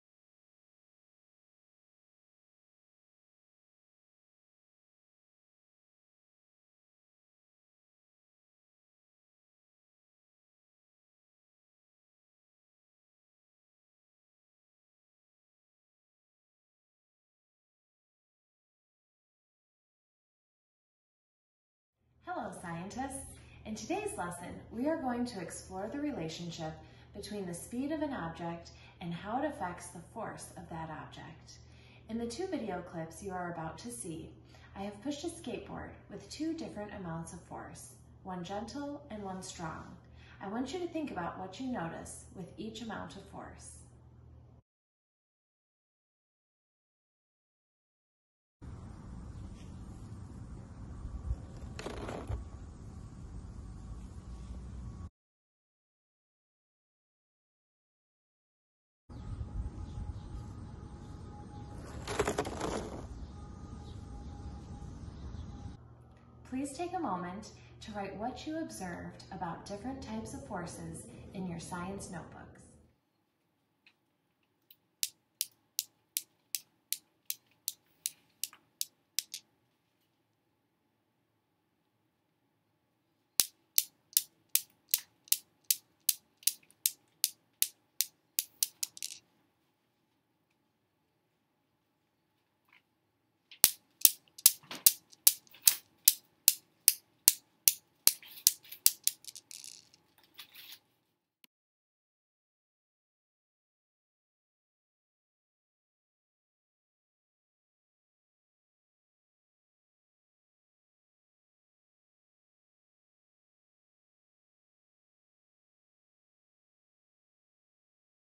My Movie 1
Watch this video and think about the driving question: How can we predict the motion of an object based on the forces acting on it?